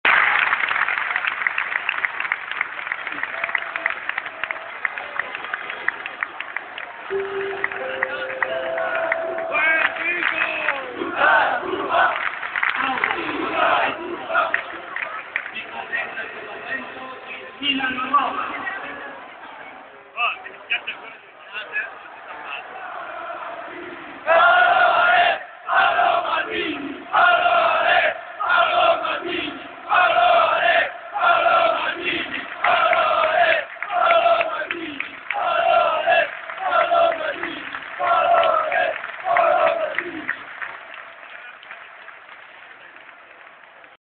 addio di maldini milan roma .
coro per il capitano prima del fischio d'inizio.